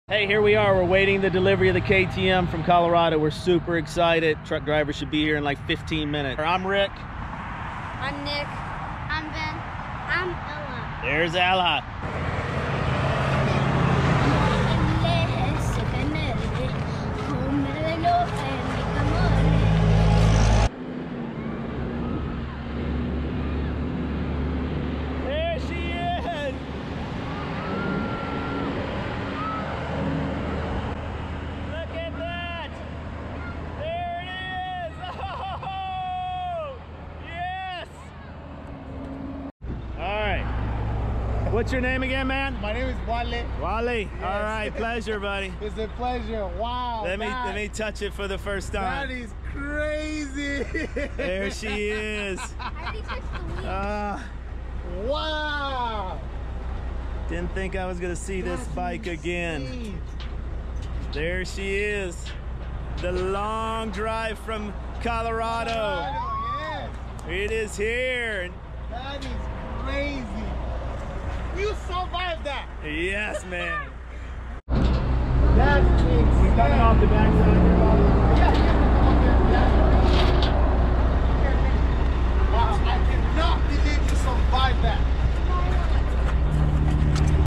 Part #3 Cliff diving KTM 300 XC-W returns home to Texas
After a week long recovery and another week in transit, my KTM arrives back home to Liberty Hill Texas.